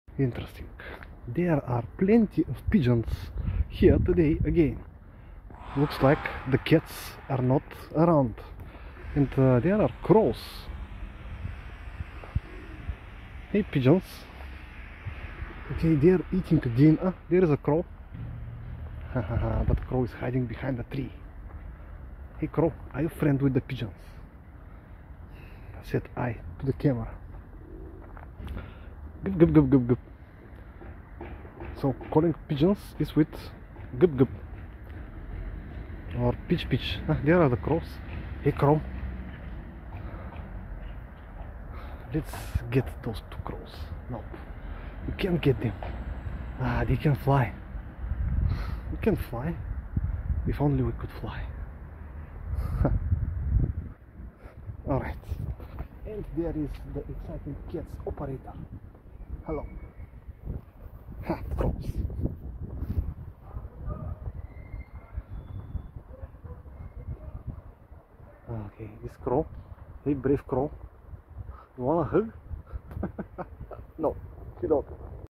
For the Birds - Adventuring 🤠
A walk among city birds.